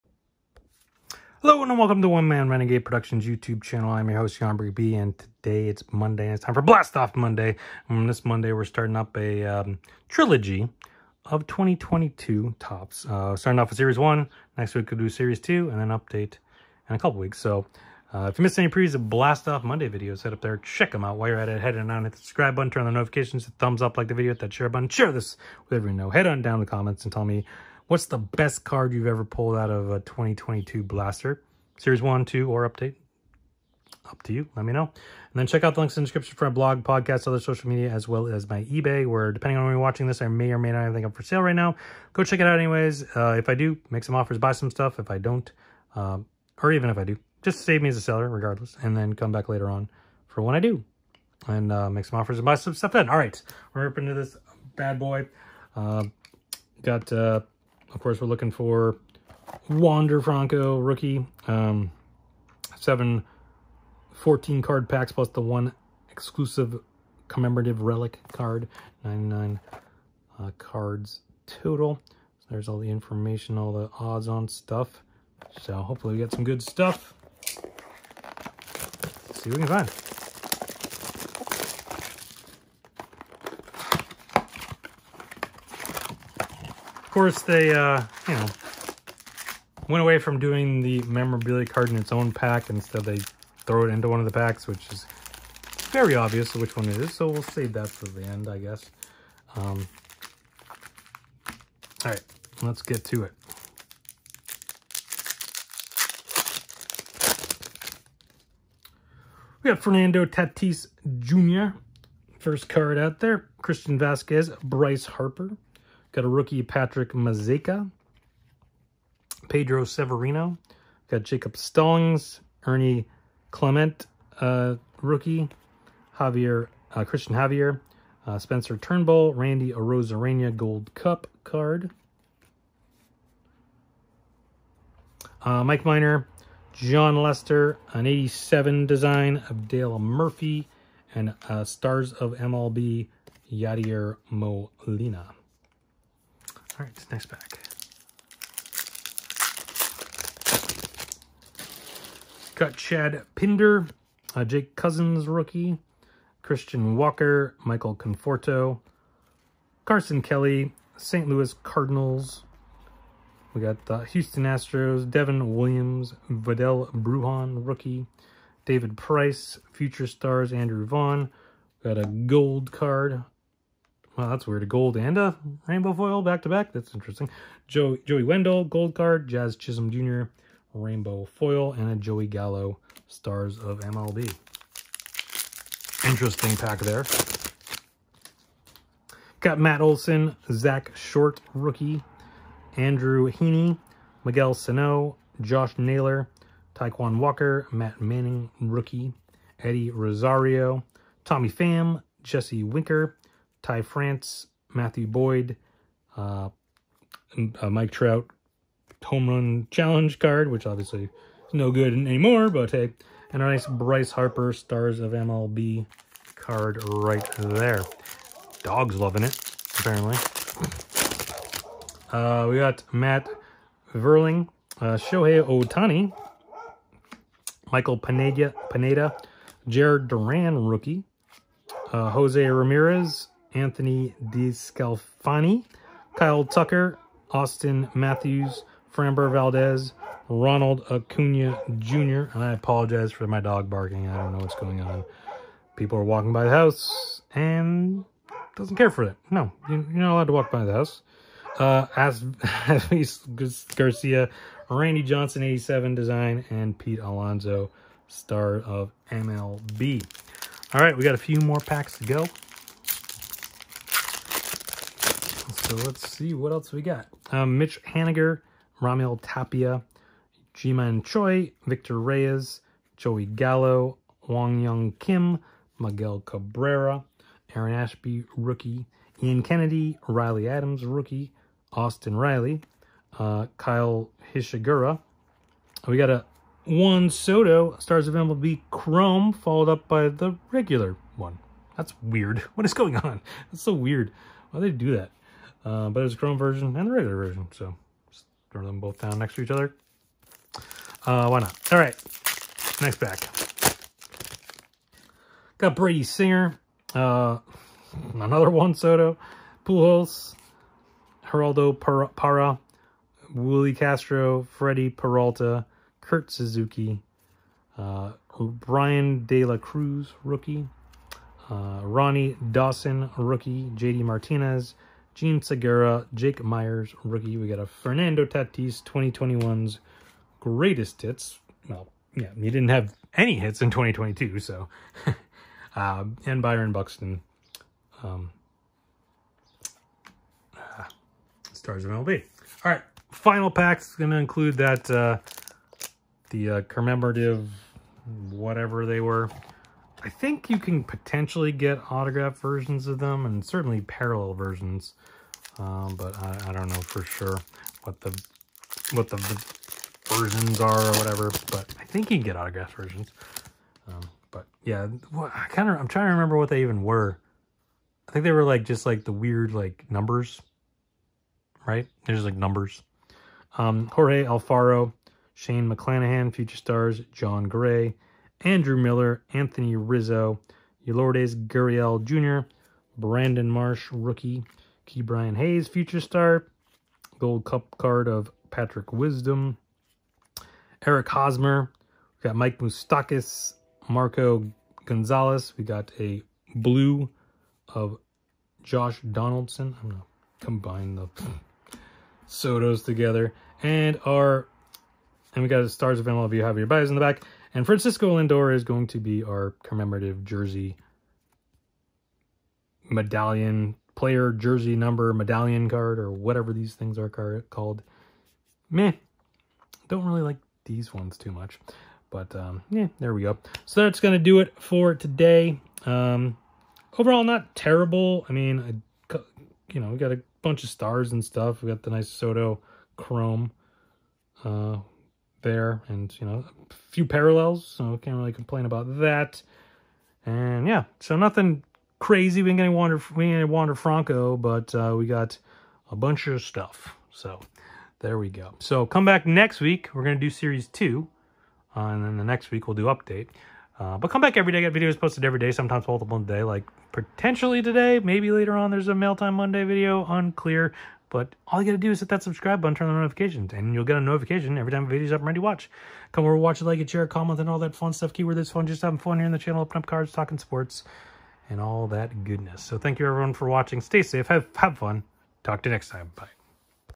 Blast-Off Monday: 2022 Topps MLB Baseball Series 1 Blaster Box! ⚾
Blast Off Monday continues with a blaster box of 2022 Topps Series 1 Baseball! Check it out to see if I can pull any of the big rookies and then come back next week for another Blaster Box!

Facebook: One Man Renegade Podcast